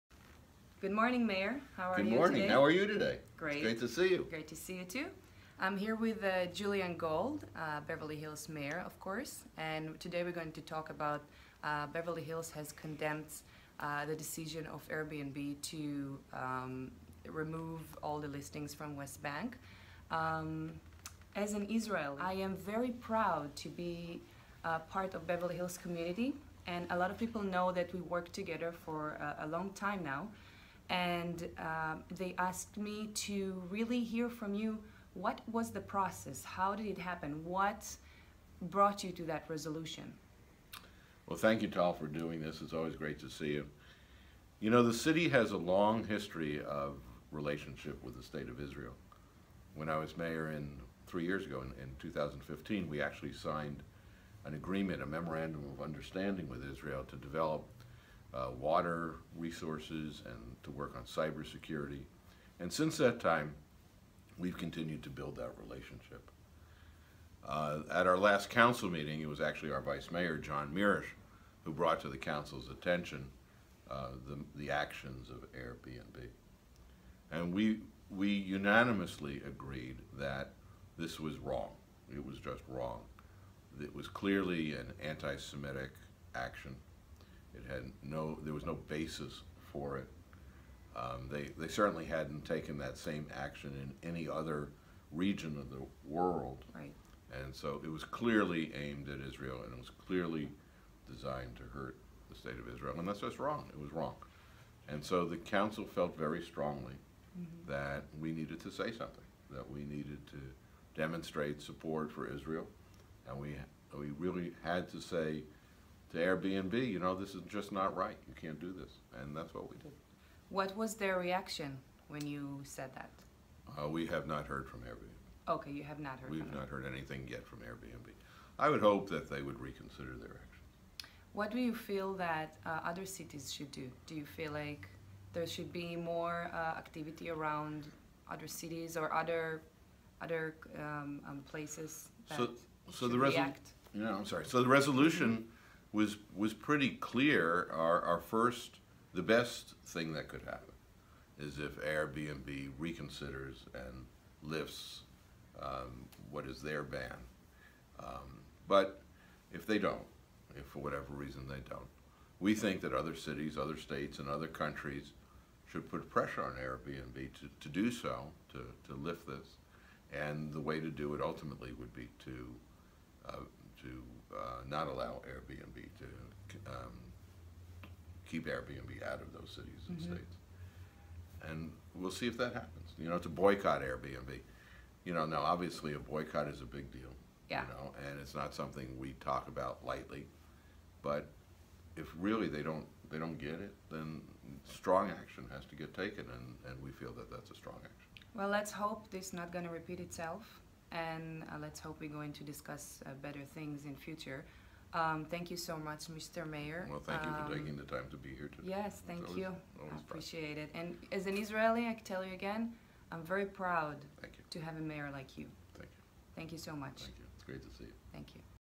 Beverly Hills Mayor Julian Gold Interviewed By Digital Marketer Tal Navarro
Tal Navarro interviews Beverly Hills Mayor, Dr. Julian Gold about the Beverly Hills city council decision to condemn Airbnb removal of listings at the West Bank.